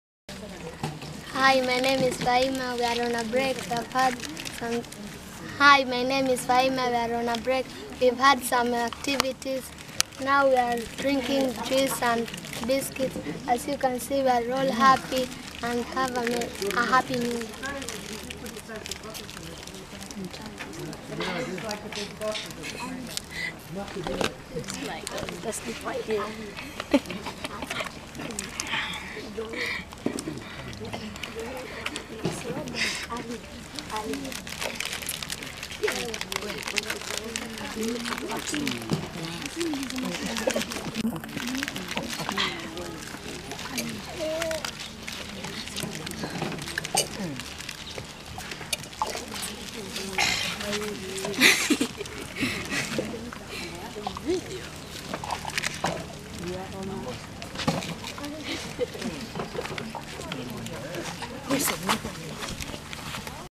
Active day of fun
​In our final visit to the New Year's Eve sports day at Happy House, we join the children during a break.
They are recharging their batteries with juice and biscuits before getting back on the field for a second  half of activities.
What happy, healthy kids we have at Happy house and how different their lives would have been without Mama Sue and the charity she founded.
We have so much to be thankful for.
*The event started with prayers of thanksgiving, led by Mama Sue.